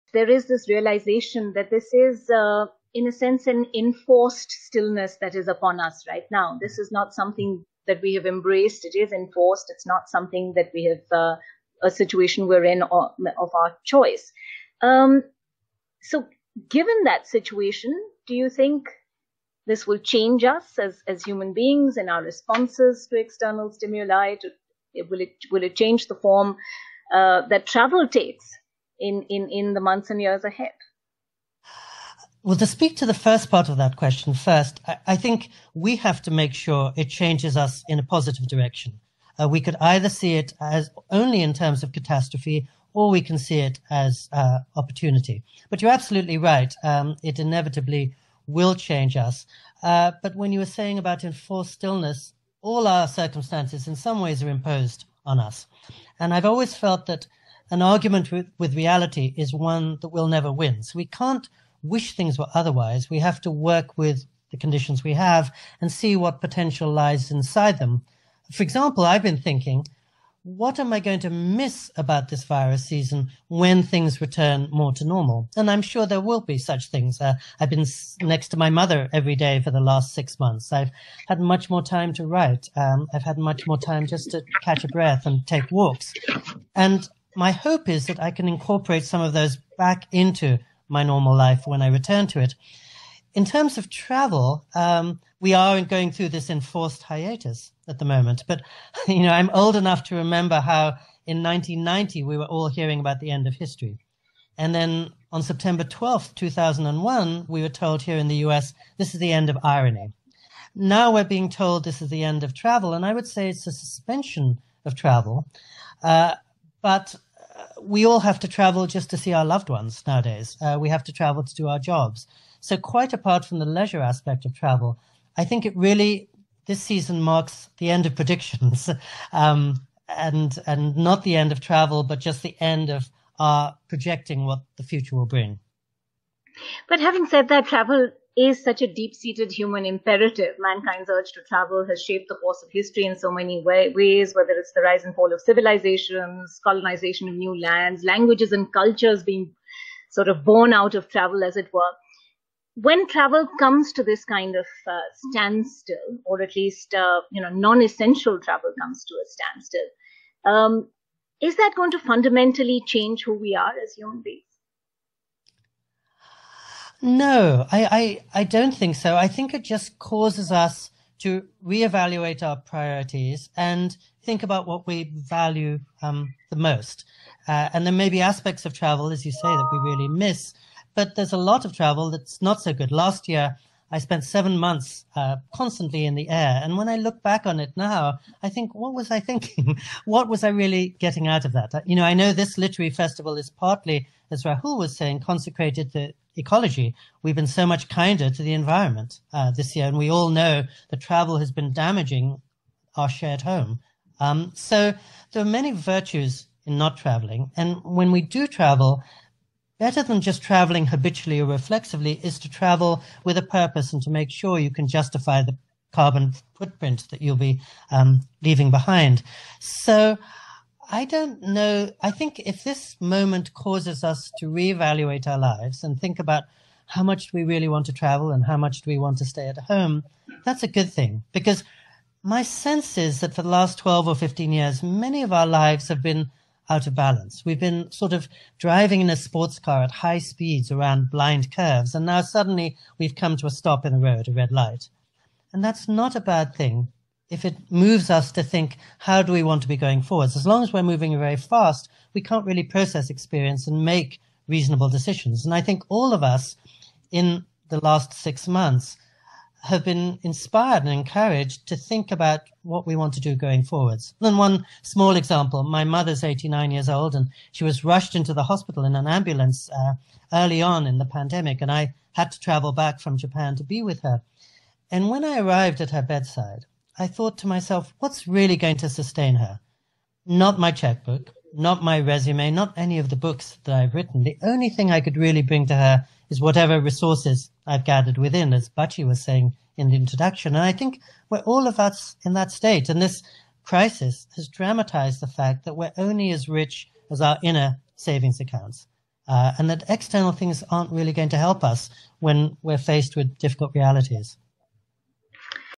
"This Season Marks The End Of Prediction": Essayist Pico Iyer On Covid-19
Essayist and author Pico Iyer talks of how the hiatus forced by COVID-19 will bring a change to his normal life when he returns to it. He said the pandemic has caused people to re-evaluate realities. "There are many virtues to not travelling," he said.